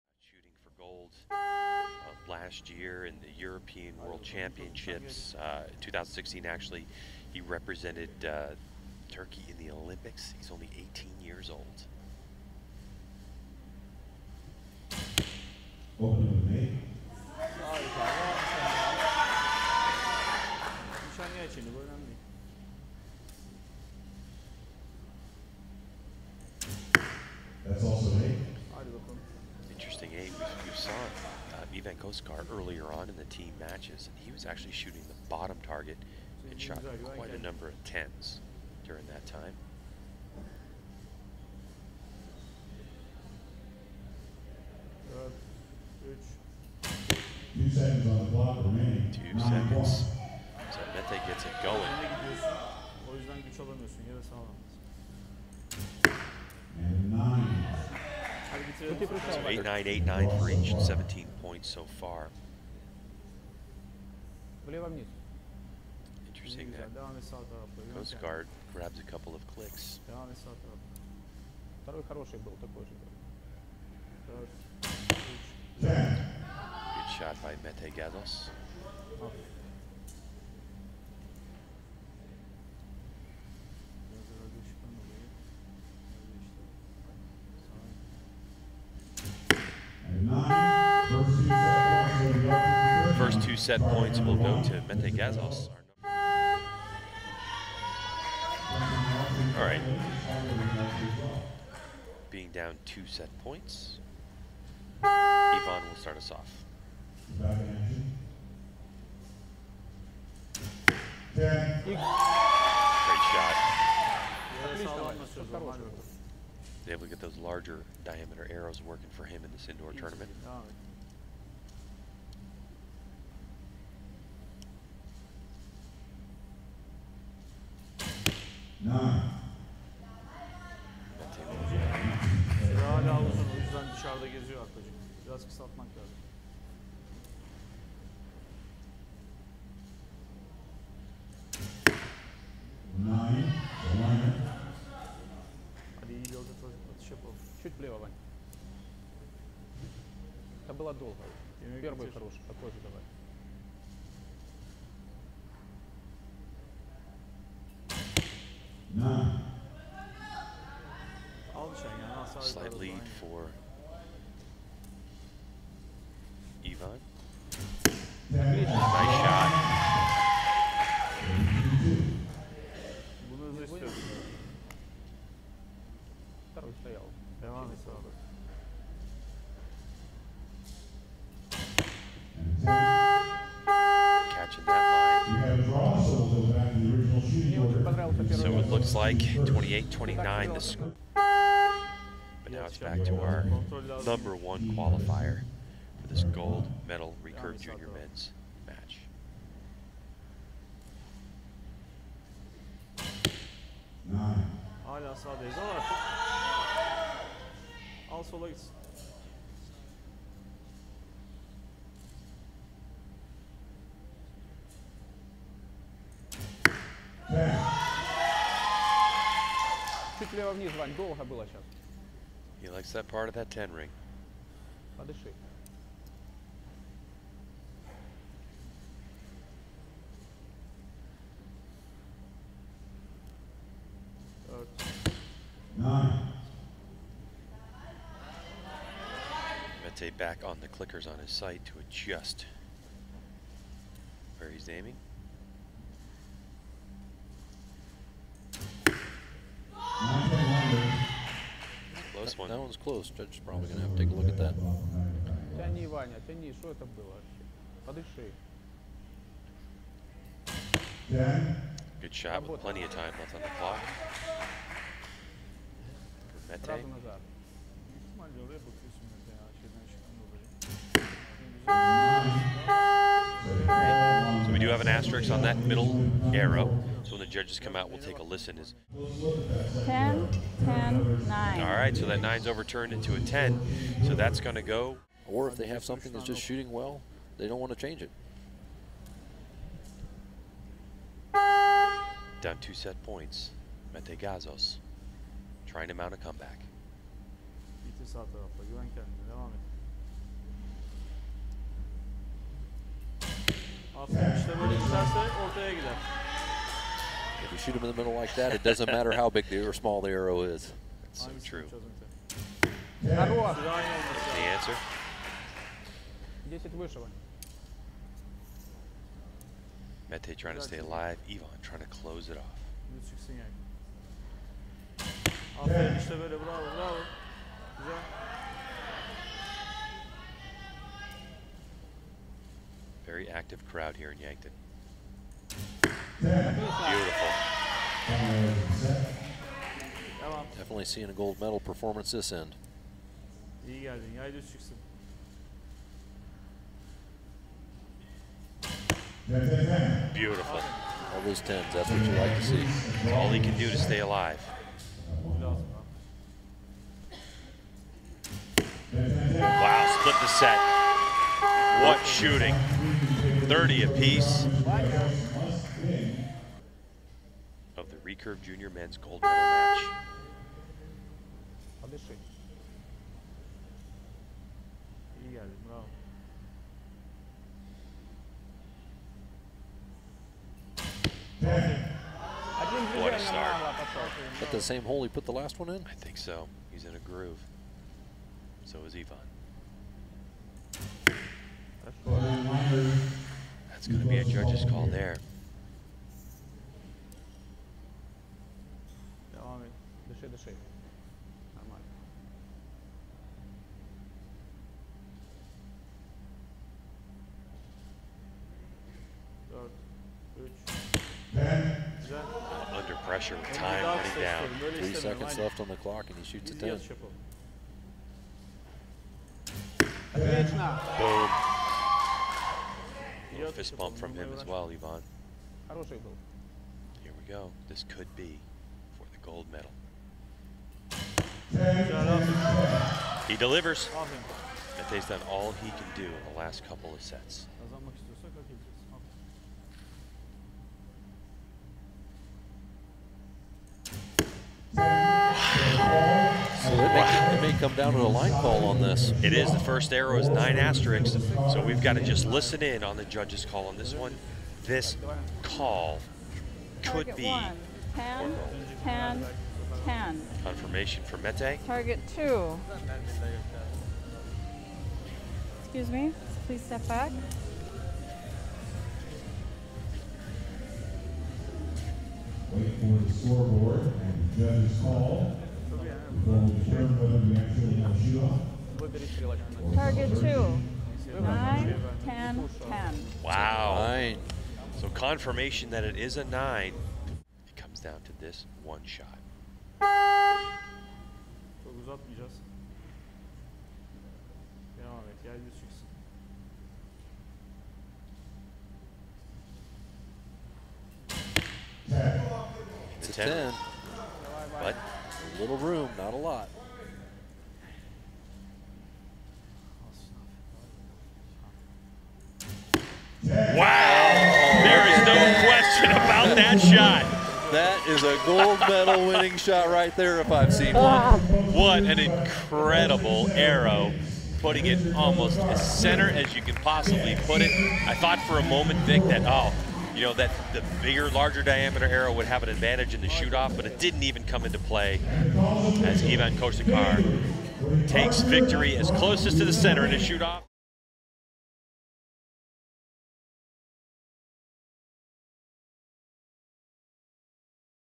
Condensed: Mete Gazoz shooting for the under-21 indoor world title in 2018
Condensed match from the 2018 World Archery Indoor Championships in Yankton, USA.